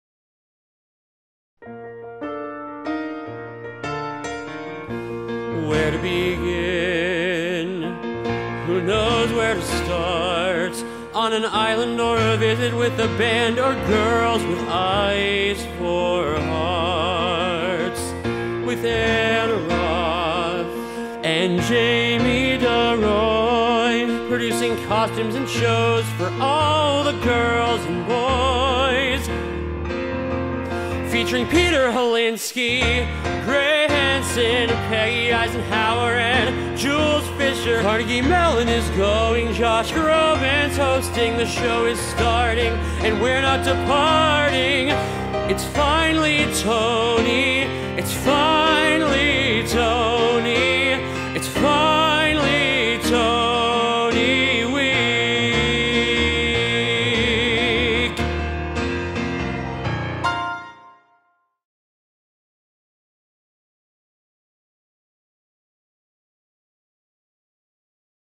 It’s Finally Tony Week!
With alumnus Josh Groban co-hosting the 72nd Tony Awards and alumni Grey Henson, Ann Roth, Jamie deRoy, Peggy Eisenhower, Jules Fisher and Peter Hylenski nominated for Tony Awards, we could sing! Jacob Randall Holmes, graduate student in the School of Music, lent his talent and voice to help us celebrate.